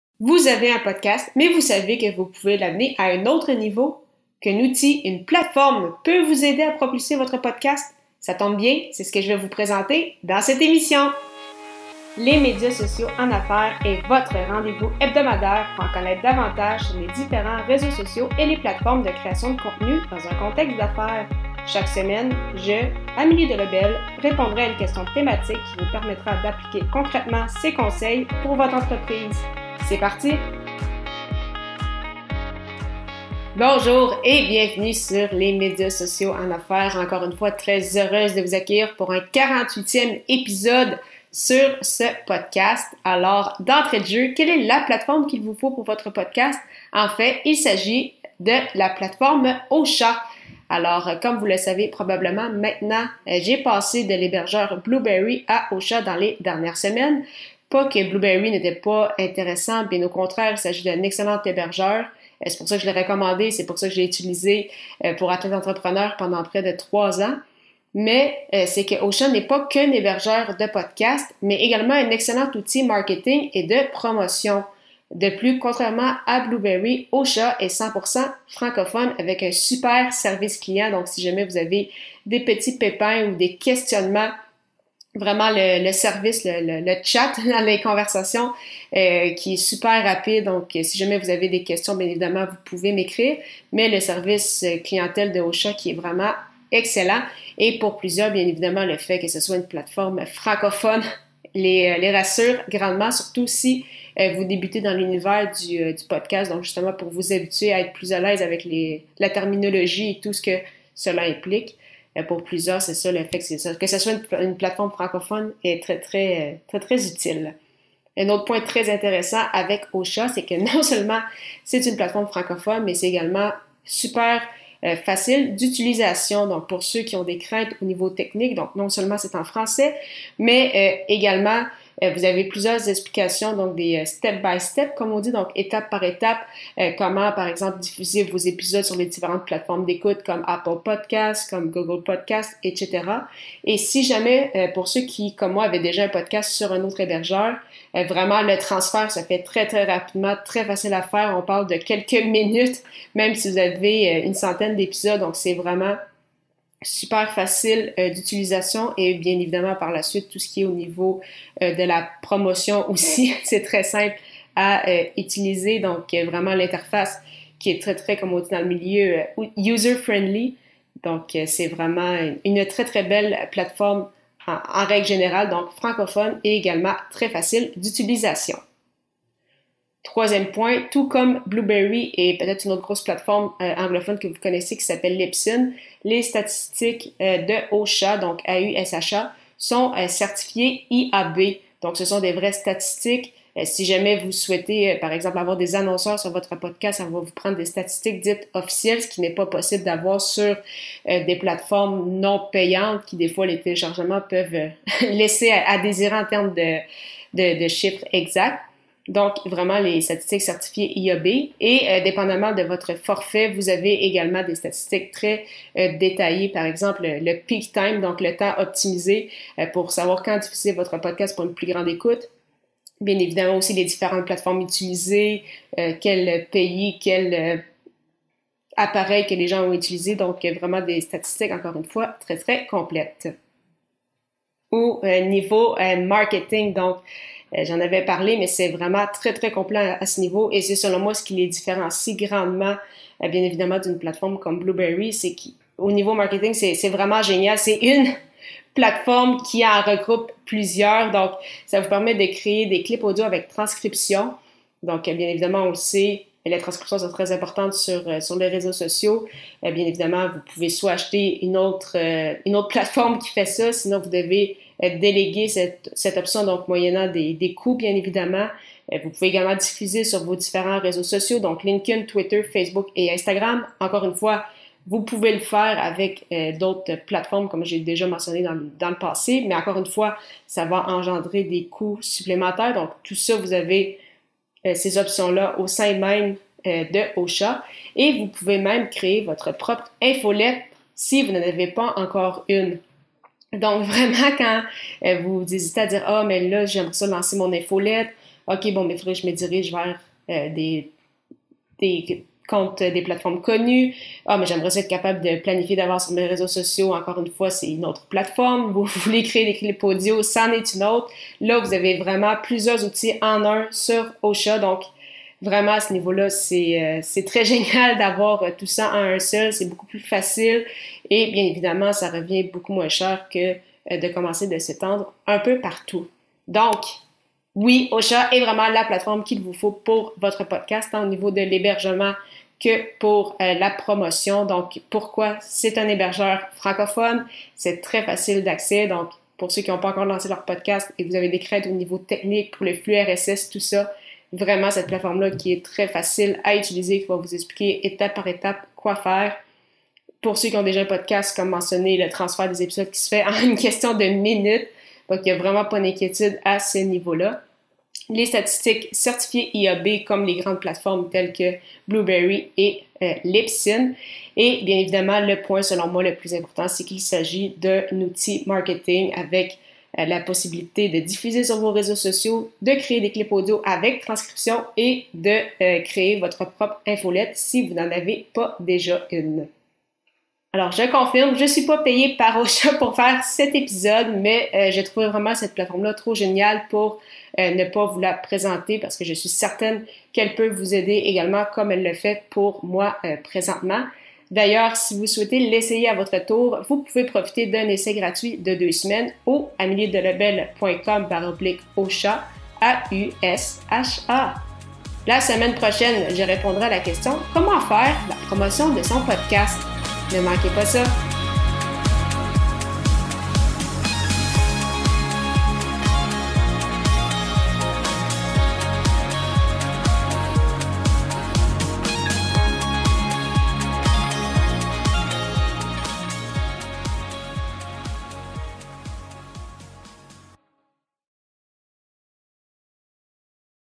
Quelle est LA plateforme qu’il vous faut pour votre podcast ? | E048
Lors de ce 48e épisode de mon podcast Les Médias Sociaux en Affaires, je réponds à la question « quelle est LA plateforme qu’il vous faut pour votre podcast ? ».

Vous avez un podcast, mais vous savez que vous pouvez l’amener à un autre niveau?

Qu’un outil, une plateforme peut vous aider à propulser votre podcast?

Ça tombe bien, c’est ce que je vais vous présenter dans cette émission!

Quelle est LA plateforme qu’il vous faut pour votre podcast ? : un outil pour vous
Je confirme, je ne suis pas payée par Ausha pour faire cet épisode! Cependant, je trouvais cette plateforme trop géniale pour ne pas vous la présenter, car je suis certaine qu’elle peut vous aider également.

Pour l’essayer à votre tour, profitez d’un essai gratuit de deux semaines au

La semaine prochaine, je répondrai à la question «  comment faire la promotion de son podcast ? ». Ne manquez pas cette 49e émission des Médias Sociaux en Affaires!